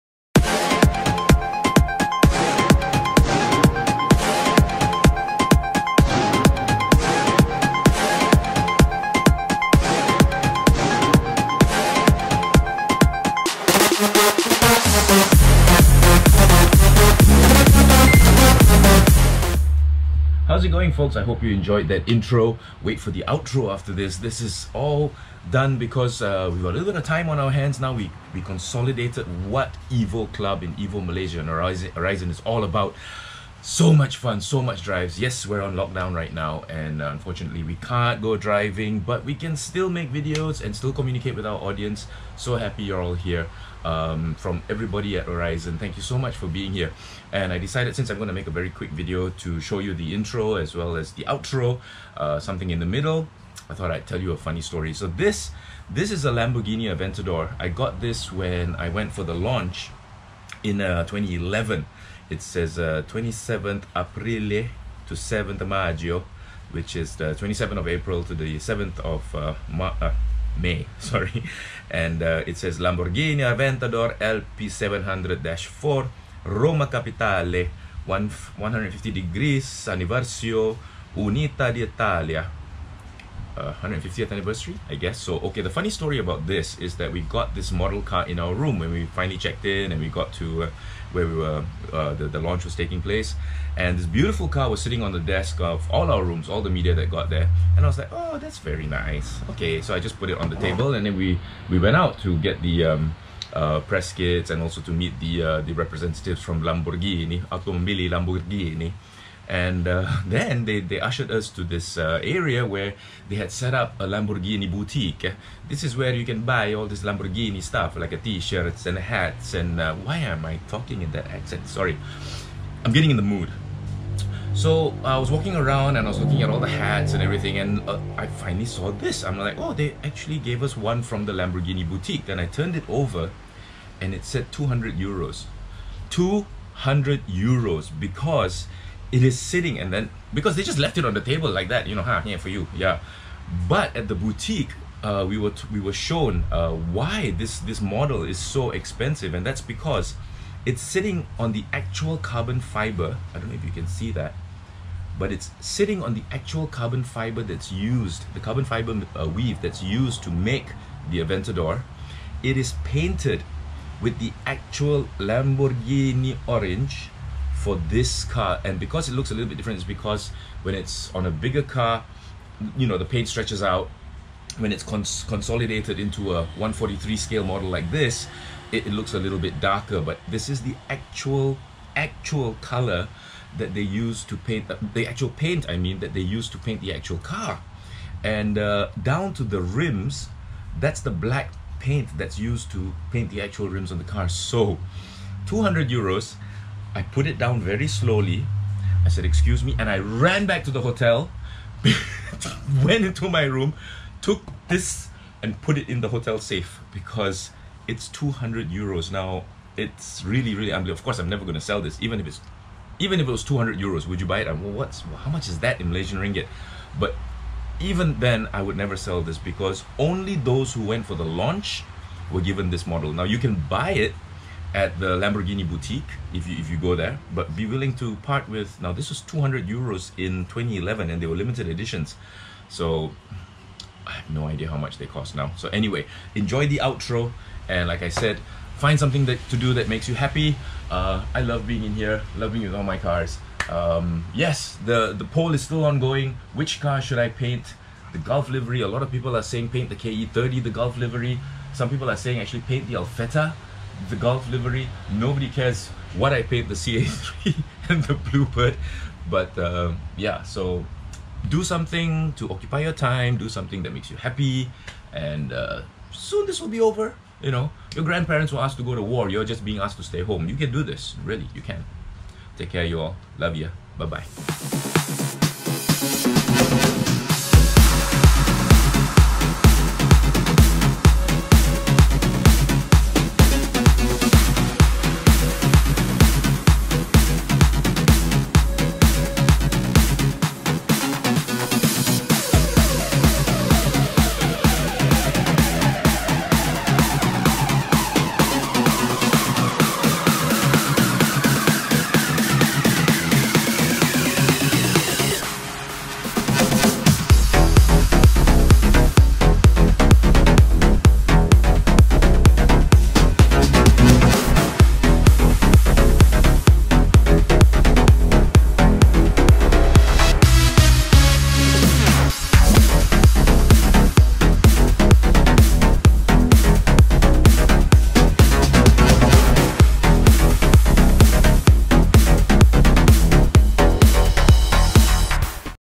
DAY 8 - SPECIAL UPLOAD: EVO Aurizn NEW Intro & Outtro + A Funny Story from ROME!! | EvoMalaysia.com
IT'S MY 1st YEAR ANNIVERSARY with Evo Malaysia!! And YES, we have a new INTRO and, err, OUTTRO for our videos too!! How do you like it? It's been a FANTASTIC year for me, a BIG shout out to my colleagues at Evo Malaysia, you're all amazing ppl, love you all!! :)

Well folks, I really wanted to show you this new video add-on, so I decided to tell y'all a funny story in the middle, cos it felt a bit weird just uploading the intro followed by the outtro, lol! ;)

True story too, you should have seen me belting down the streets of Rome when I realised the treasure I had, hahahaha!! Hope you like it! :)